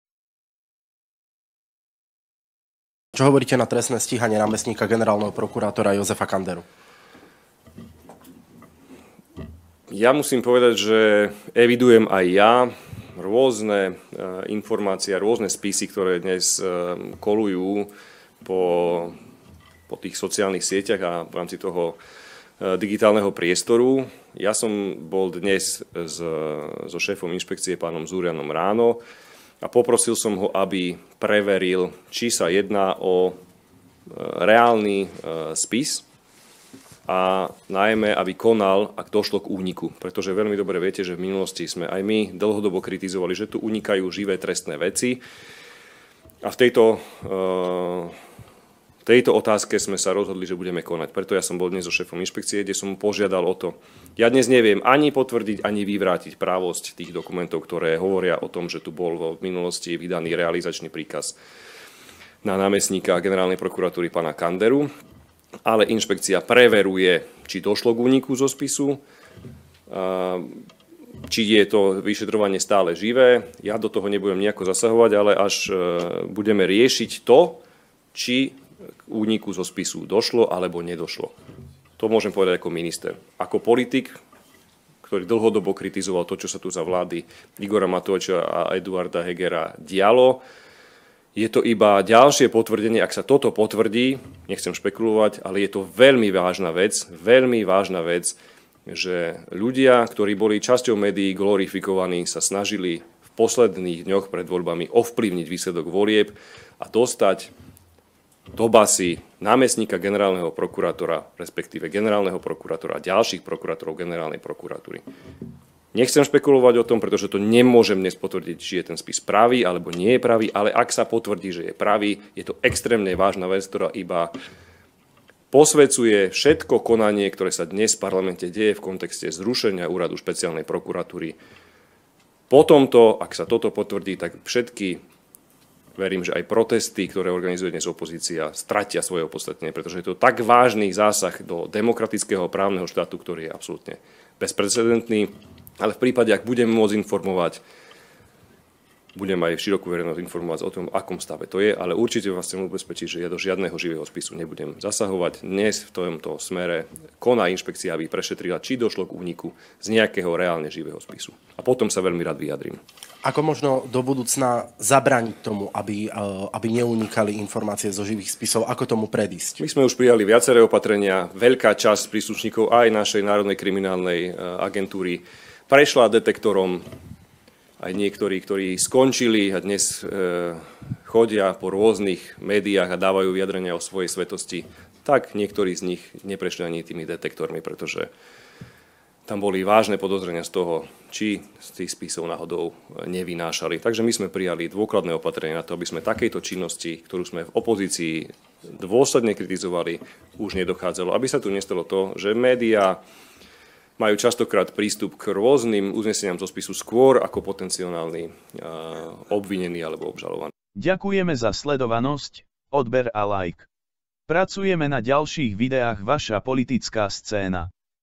Šutaj Eštok: Toto je veľmi vážne ! Únik dôležitých informácií zo spisu !! Zurian začal konať !!
Matúš Šutaj Eštok na tlačovej konferencií ( Brifingu ). Šutaj Eštok: Toto je veľmi vážne ! Únik dôležitých informácií zo spisu !!  Branislava  Zuriana poveril aby začal vo veci konať !!

Politická scéna je politický zameraný youtube kanál prinášajúci informácie zo slovenskej politiky.

Ďakujeme